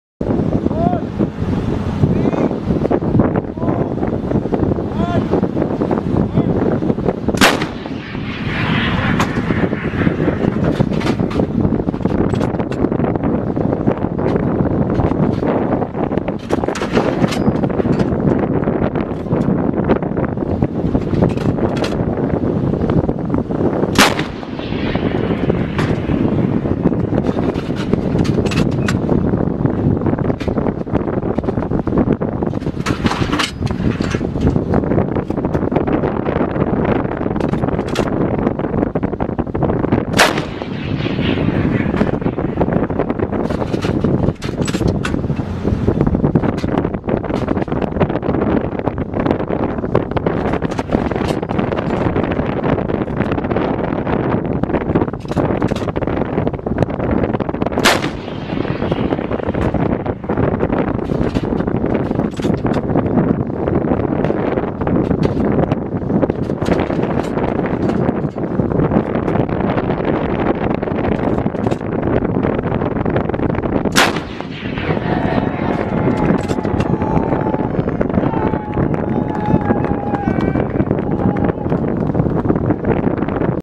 DRDO ATAGS Roaring At Thar Desert
The Advanced Towed Artillery Gun System (ATAGS) is a towed 155 mm/52 calibre howitzer that is being developed for the Indian Army by Armament Research and Development Establishment (ARDE), Tata Advanced Systems and Kalyani Strategic Systems. Bharat Forge, Tata Power SED and Advanced Weapons and Equipment India are manufacturing partners.